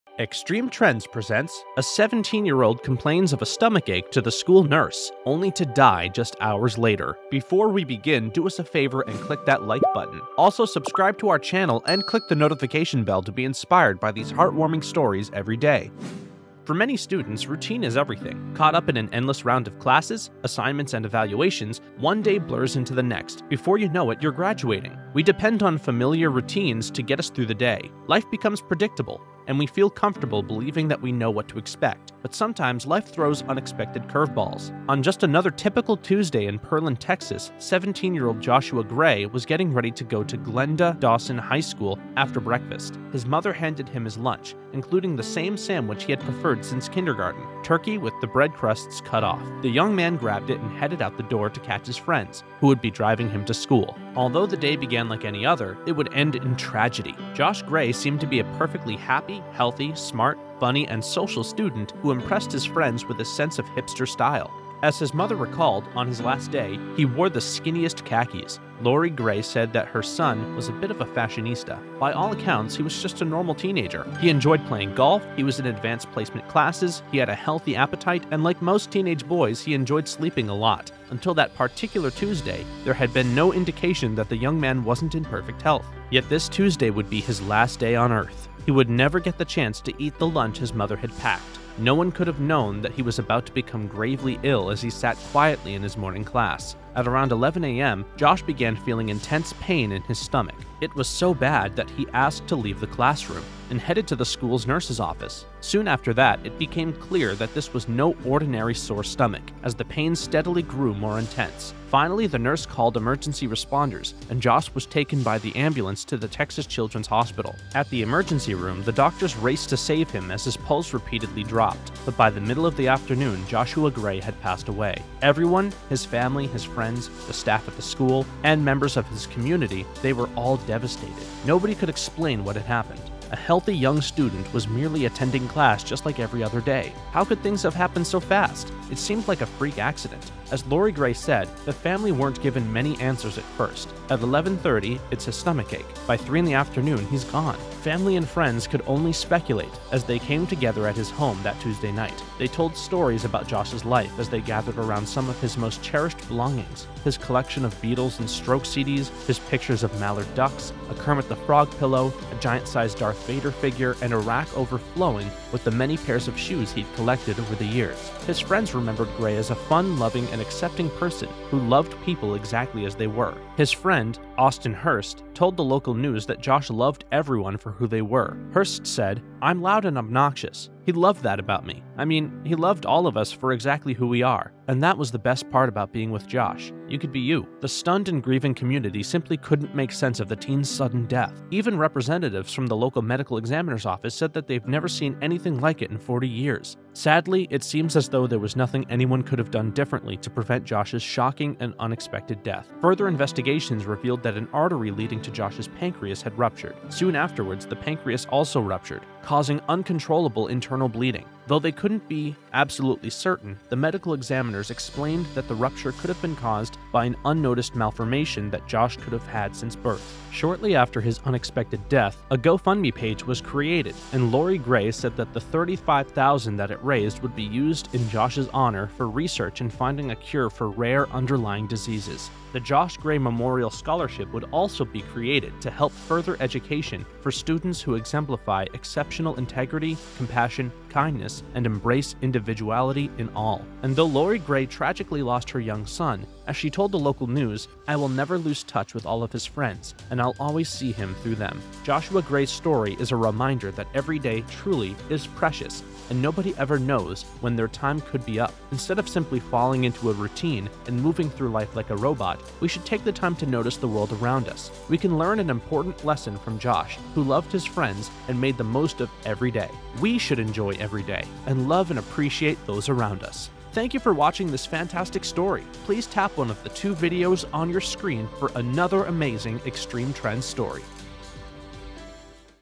17-Year-Old Complains Of Stomach Ache To School Nurse, Only To Die Just Two Hours Later
17-Year-Old Complains Of Stomach Ache To School Nurse, Only To Die Just Two Hours Later

Extreme Trends is the #1 place for all your heart warming stories about amazing people that will inspire you everyday.